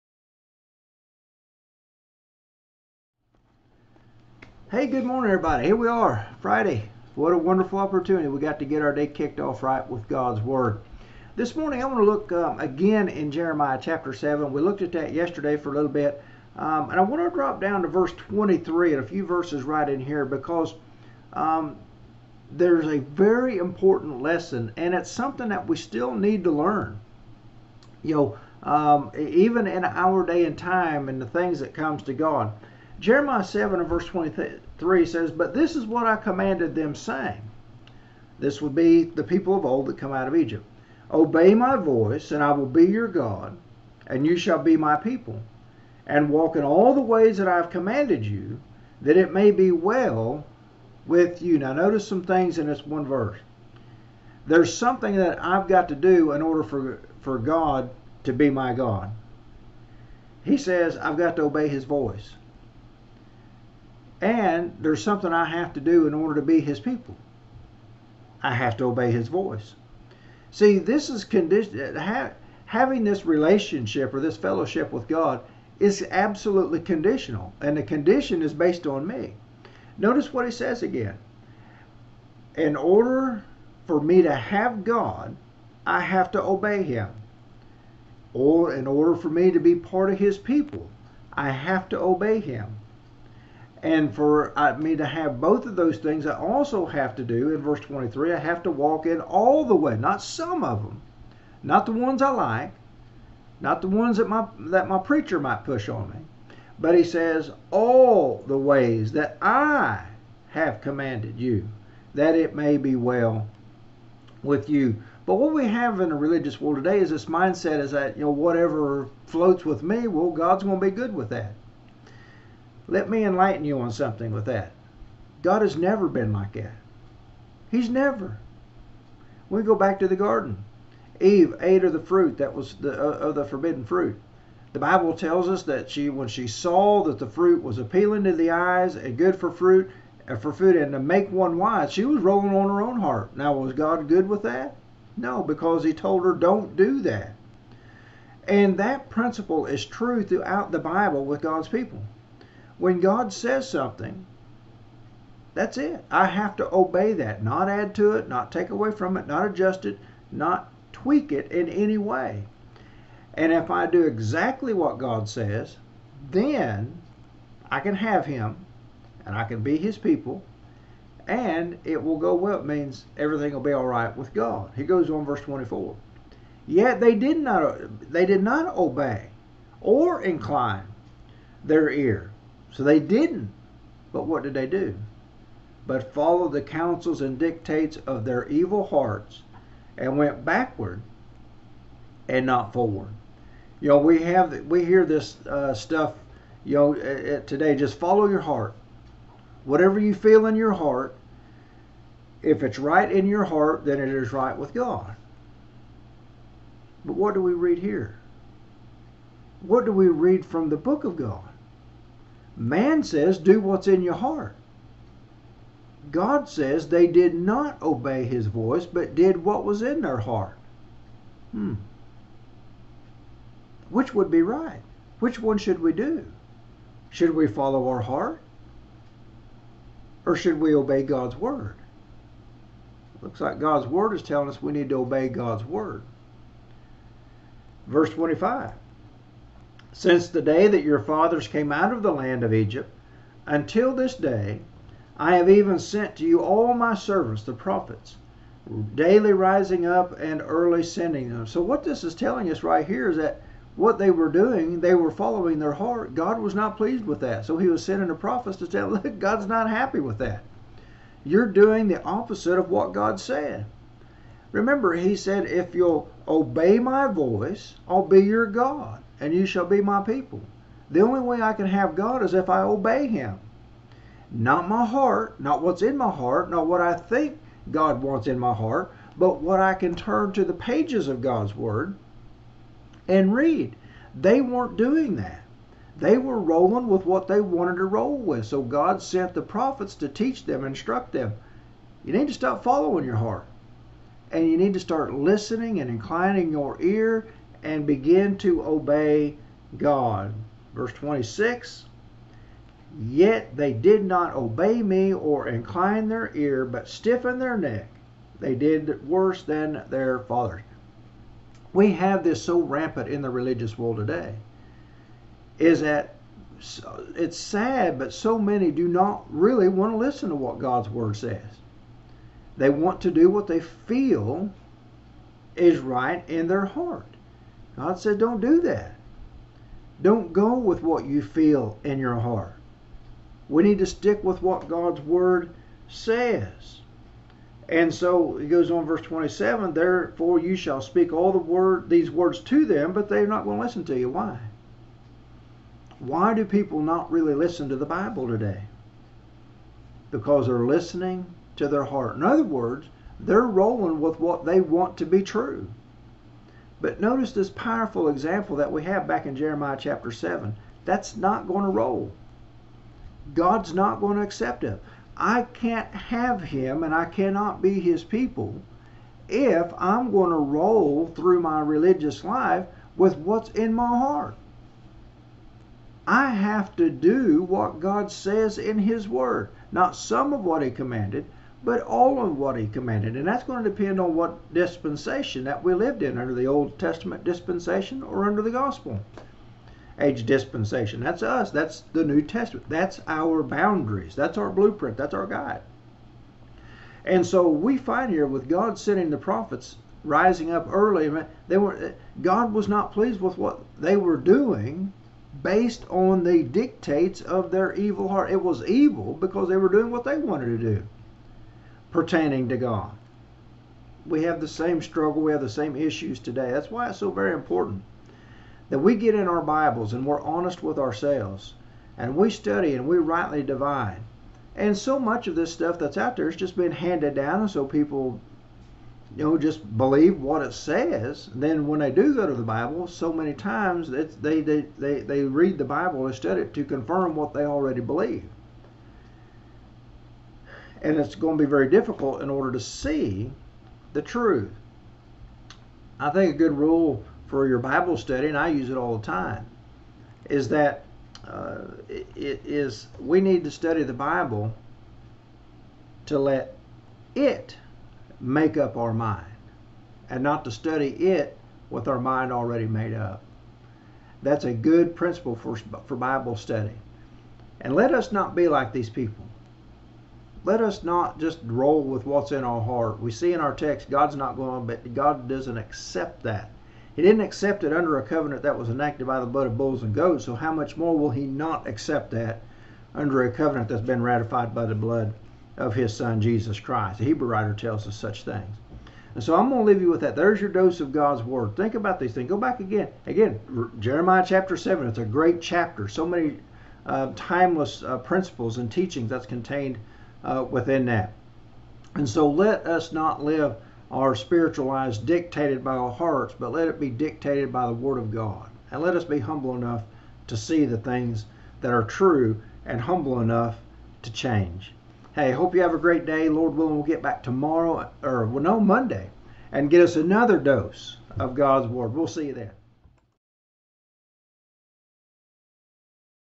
Jeremiah 7:23-27
"You're Being Lied To" -- The prophet informs the people their preachers are lying to them about their acceptance to God. They are living in ways contrary to God's will, but being told God accepts them anyway.